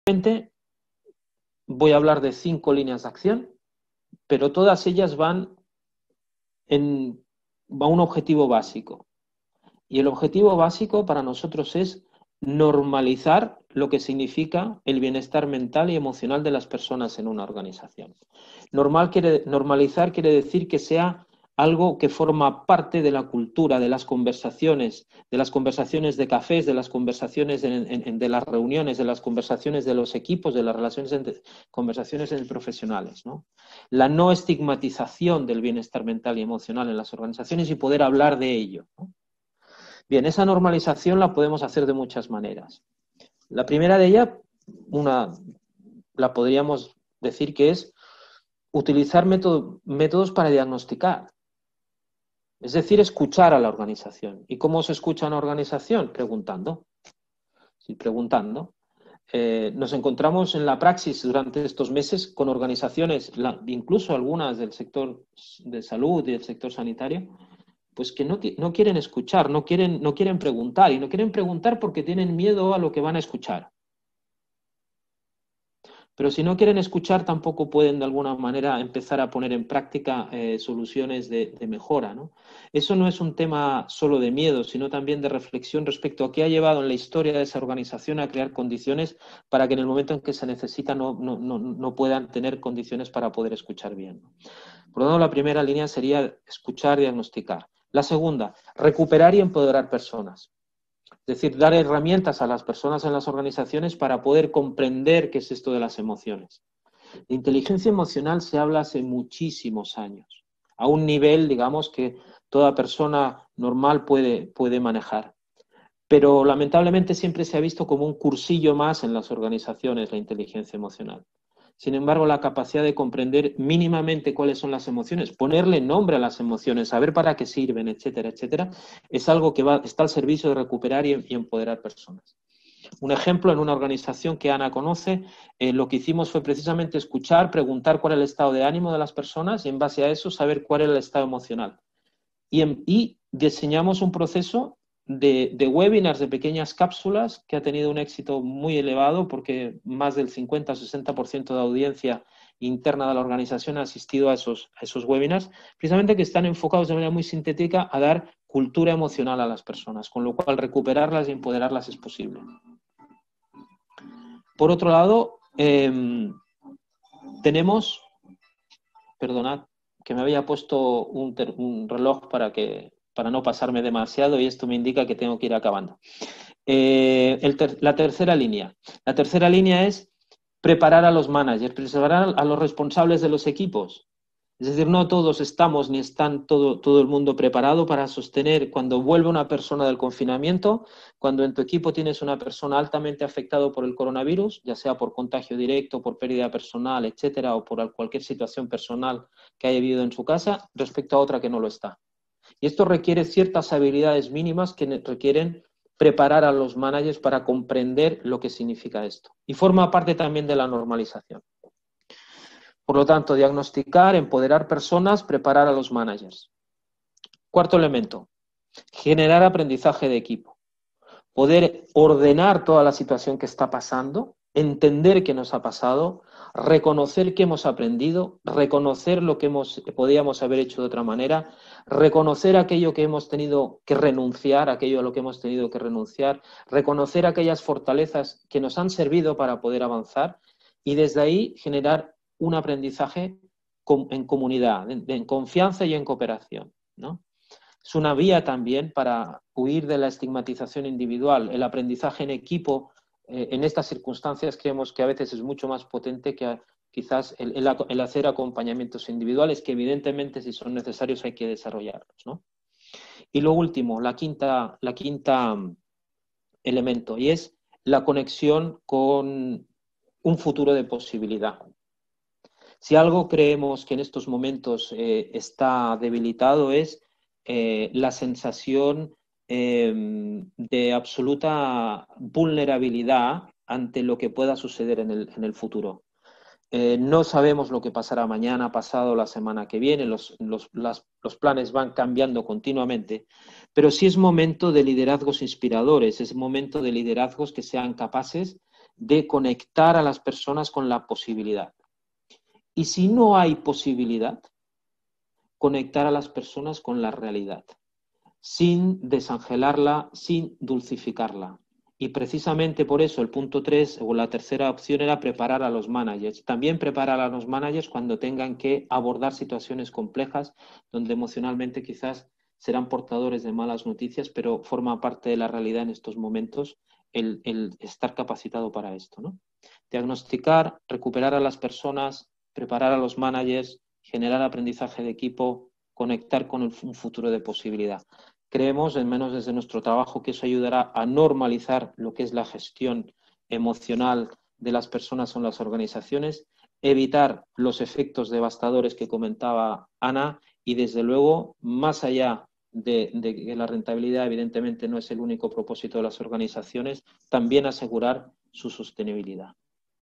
Claudio Drapkin
Claudio Drapkin, consultor empresarial, ha aportado al encuentro digital algunos consejos prácticos para directivos, responsables de recursos humanos y de comunicación interna para gestionar el bienestar de las personas en el entorno laboral a la vez que se normaliza la salud mental.